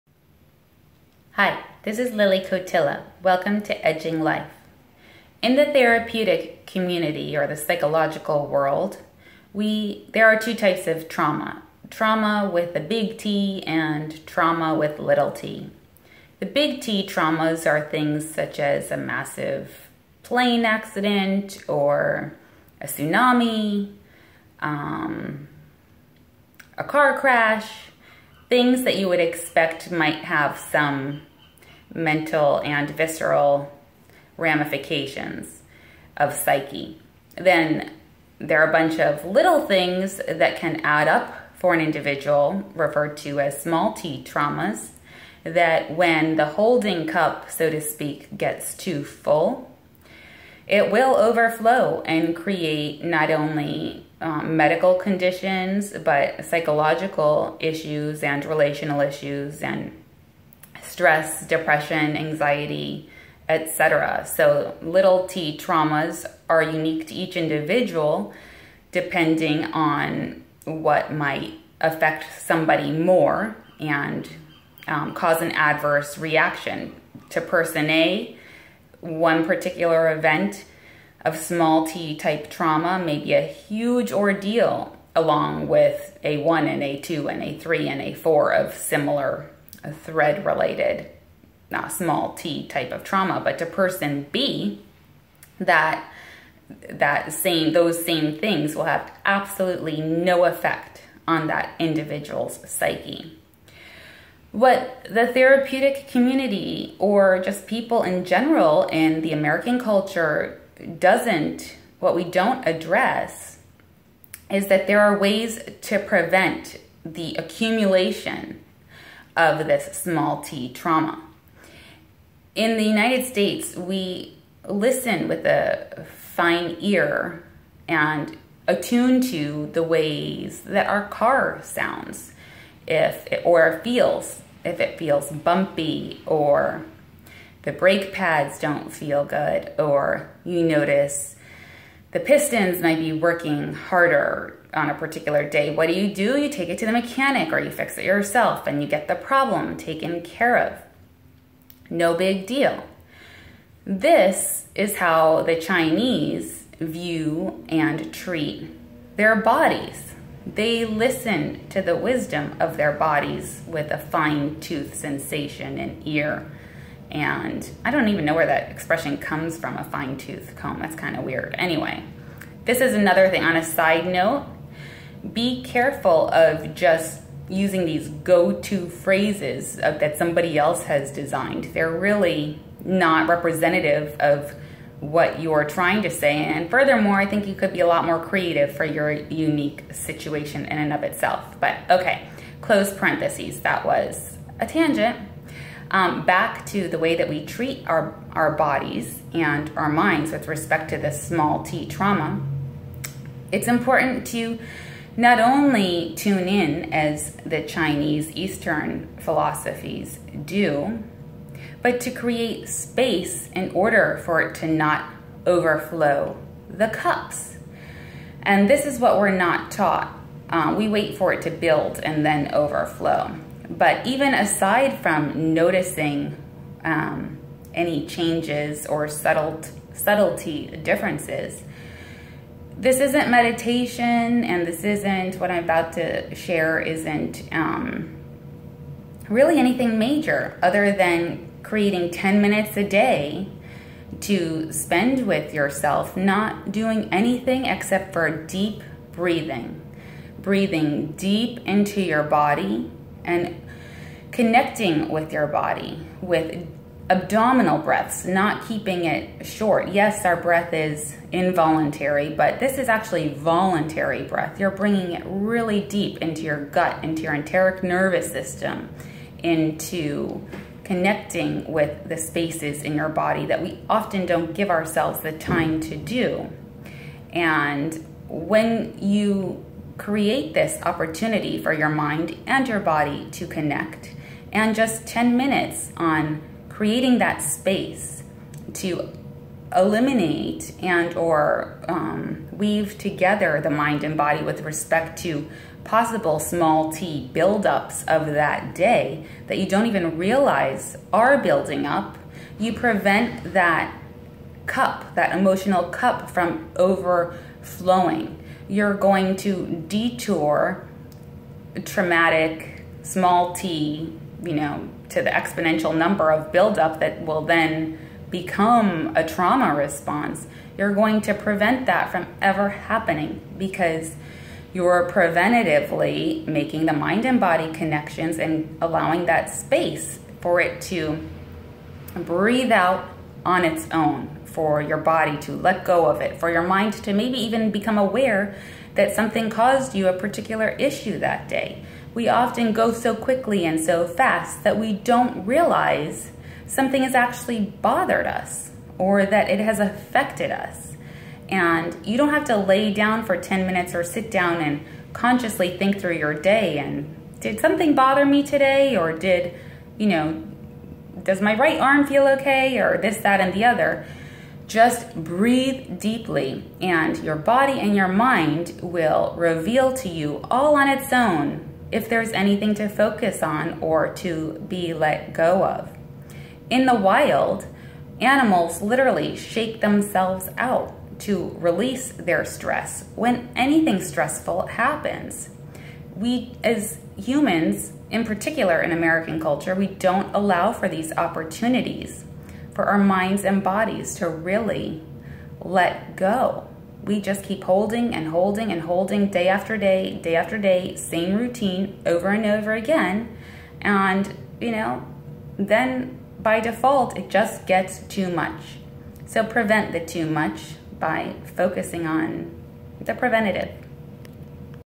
Prevent the build of little traumas
Create about 10 minutes of daily time and space to breathe deeply into your "gut," the enteric nervous system.   Breathing into your body allows for association of mind, body and experience.  It allows for mind-body connections to happen that otherwise might not integrate into subconscious or conscious awareness. Creating 'spaces of settling' and absorbing of daily stimuli is important in order to chunk, process and make sense of life, relationships and the self.  With breath, create a release valve for daily traumas of mind and body such that they don't boil up or overflow your cup of psyche.